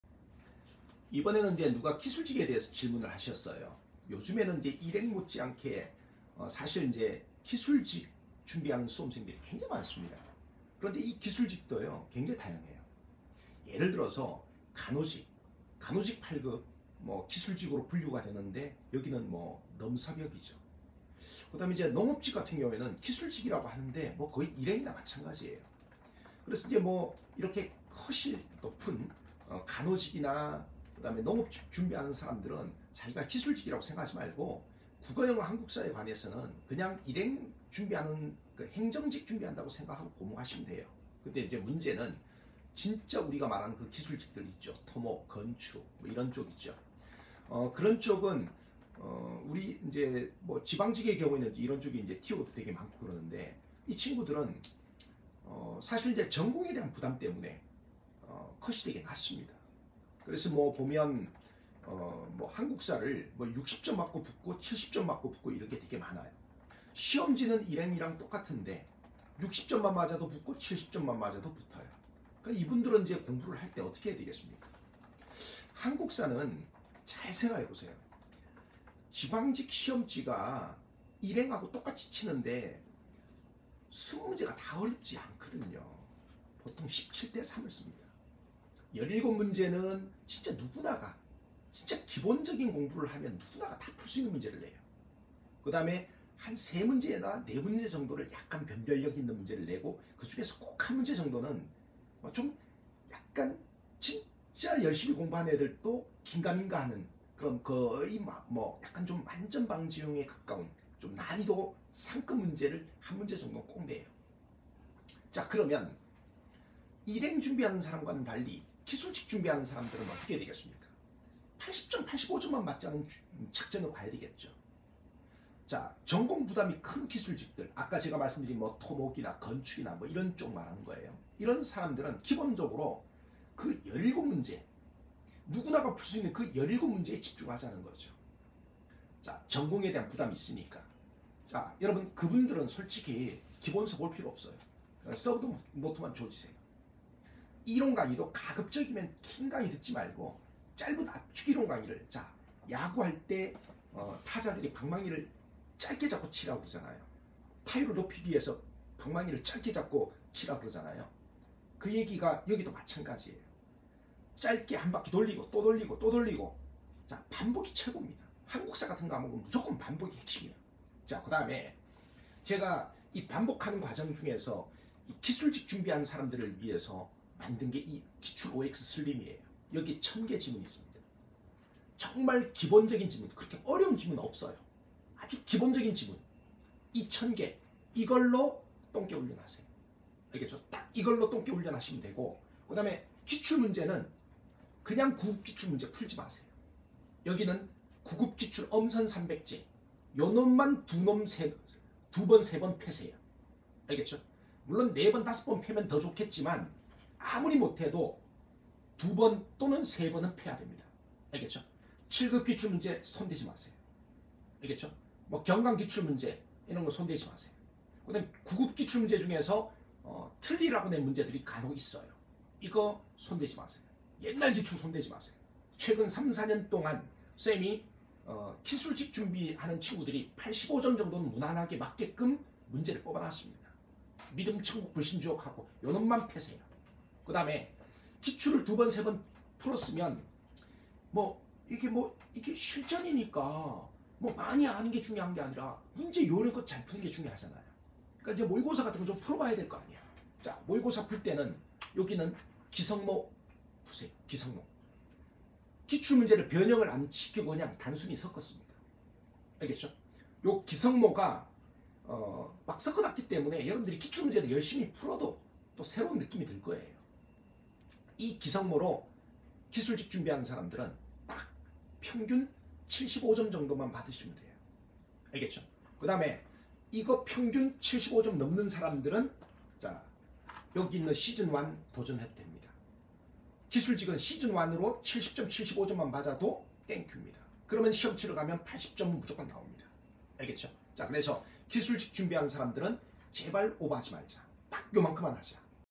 [고종훈 한국사] 기술직은 한국사 어떻게 준비하나요?
고사부 다음 카페에 올라온 질문입니다.

Q. 기술직은 한국사 어떻게 준비하나요?

매년 중복되는 질문이 많아 카페에 "자주 묻는 질문" 게시판을 신설하였습니다.
고사부 다음 카페 "문의 제안" 게시판에 문의 내용을 제안해주시면 질문을 채택하여 선생님께서 직접 영상으로 답변드릴 예정입니다.